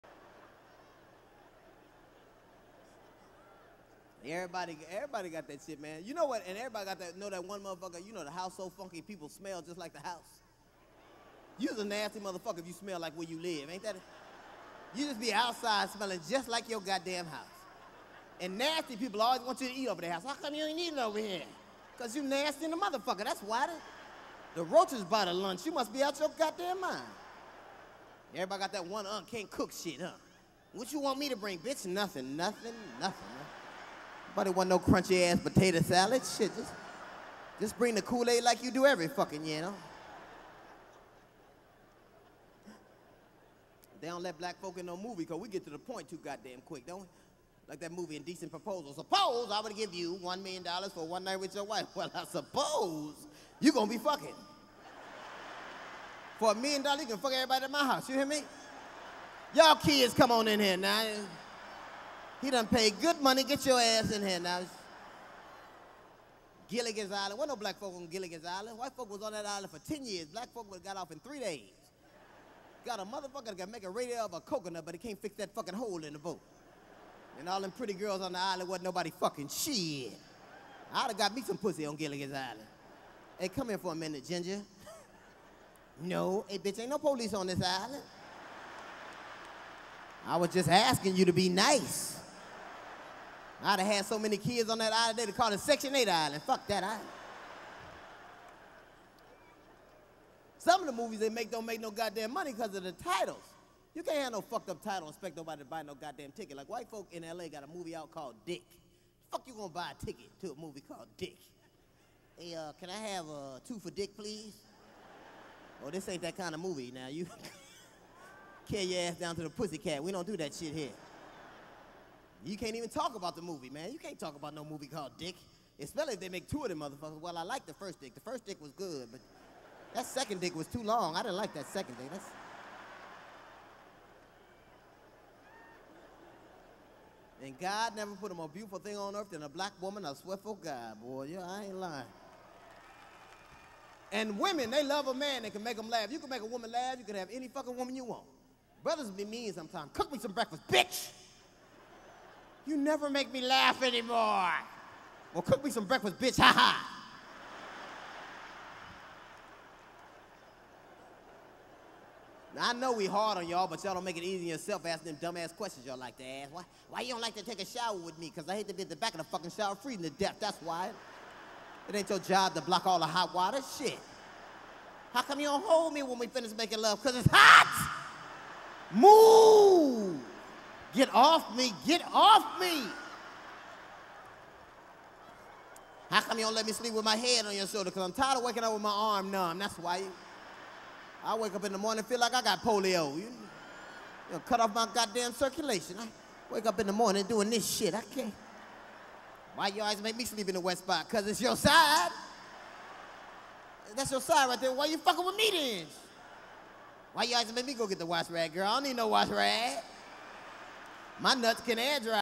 DL Hughley "The Wet Spot" Kings of Comedy Tour
Why do I have to sleep on the wet side...

"Always Original"

Walter Latham